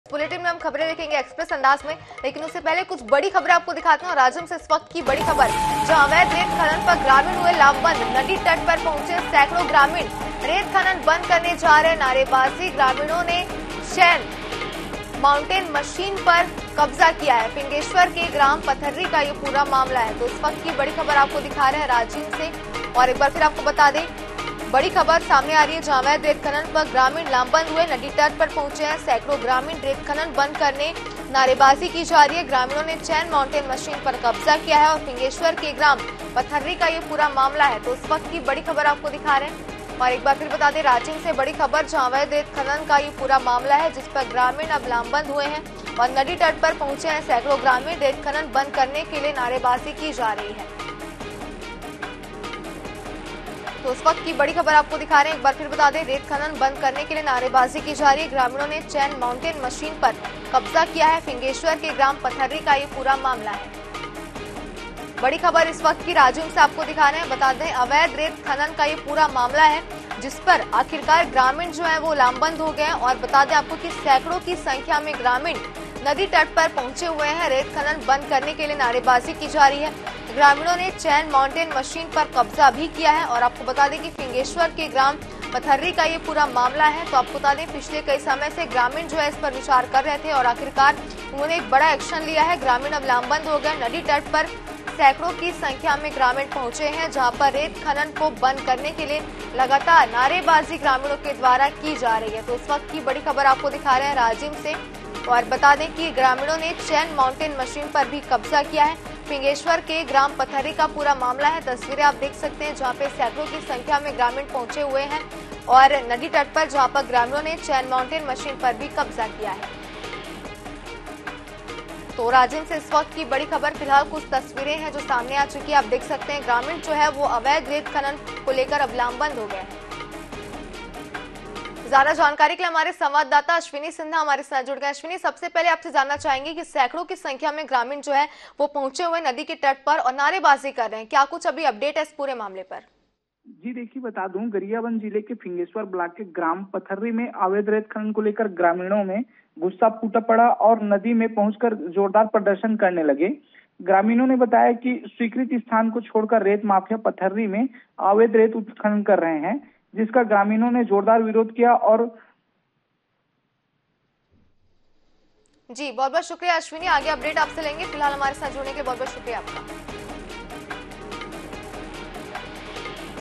Illegal Sand Mining : अवैध रेत खनन पर ग्रामीणों ने किया लामबंद, नारेबाजी करते मशीनों को करवाया बंद
INH, Illegal Sand Mining : अवैध रेत खनन पर ग्रामीणों ने किया लामबंद, नारेबाजी करते मशीनों को करवाया बंद